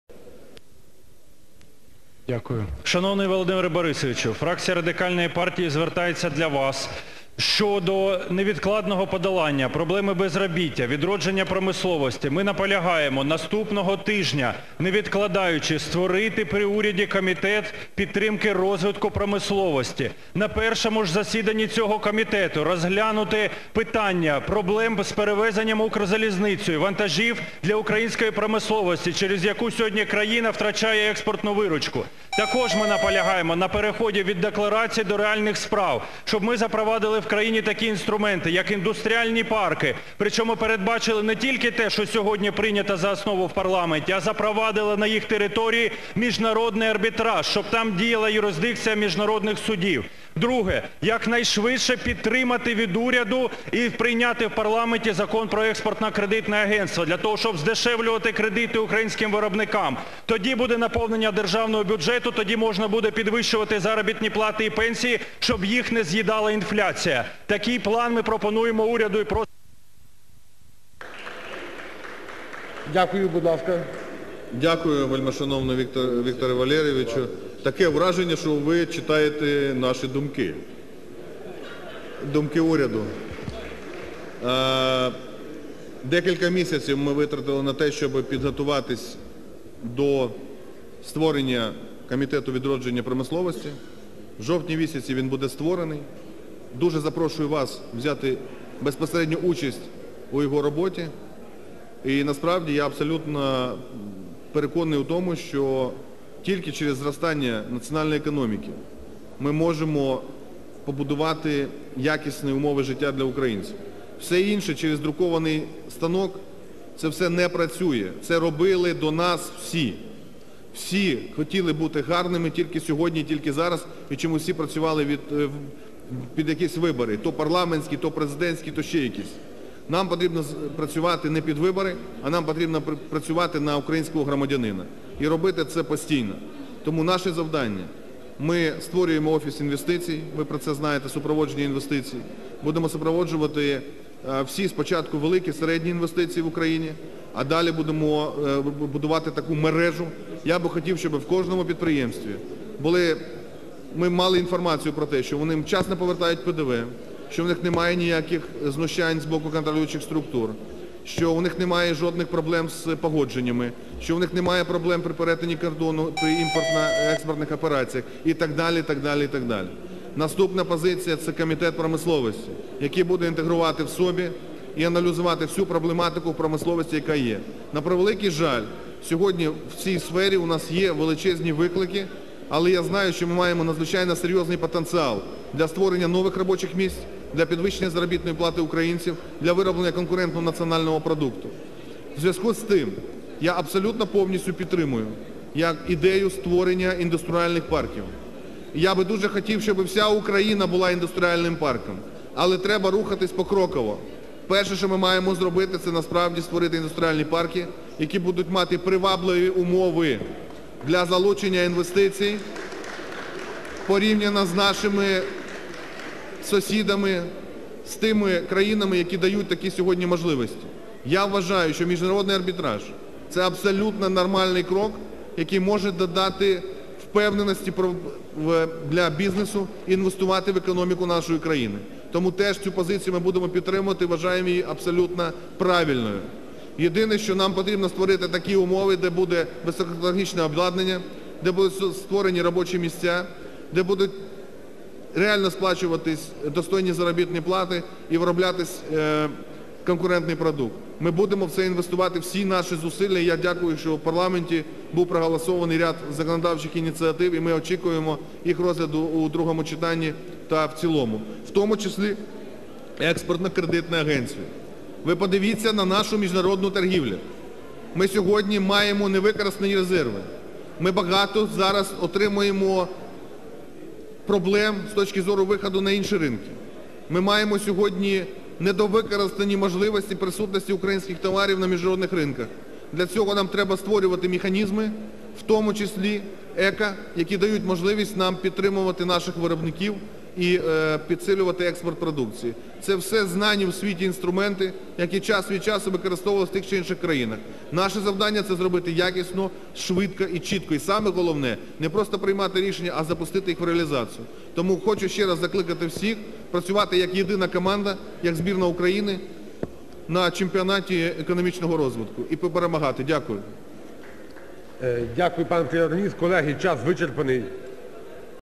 Галасюк читає думки уряду
Народний депутат України, заступник лідера Радикальної партії Віктор Галасюк під час Години запитань до Уряду наголосив на потребі невідкладного подолання проблеми безробіття та відродження промисловості.

"Ми наполягаємо наступного тижня, не відкладаючи, створити при уряді комітет підтримки розвитку промисловості. На першому ж засіданні цього комітету розглянути питання проблем з перевезенням "Укрзалізницею" вантажів для української промисловості, через яку сьогодні країна втрачає експортну виручку", - зазначив Галасюк, звертаючись до Гройсмана.

Час переходити від декларацій до реальних справ, - переконаний радикал. "Щоб ми запровадили в країні такі інструменти, як індустріальні парки, причому передбачили не тільки те, що сьогодні прийнято за основу в парламенті, а запровадили на їх території міжнародний арбітраж, щоб там діяла юрисдикція міжнародних судів", - пояснив він.

Крім цього, нардеп закликав якнайшвидше підтримати від уряду і прийняти в парламенті Закон про експортно-кредитне агентство для того, щоб здешевлювати кредити українським виробникам: "Тоді буде наповнення державного бюджету, тоді можна буде підвищувати заробітні плати і пенсії, щоб їх не з'їдала інфляція. Такий план ми пропонуємо уряду".

Відповідь Прем'єр-міністра на запитання радикала була ствердною: "Шановний  Вікторе Валерійовичу, таке враження, що ви читаєте наші думки, думки уряду. Кілька місяців ми готувалися до створення Комітету відродження промисловості. Запрошую вас взяти безпосередню участь у його роботі. Переконаний, що лише через зростання нашої економіки ми зможемо побудувати якісне життя українців, а не через друкарський станок".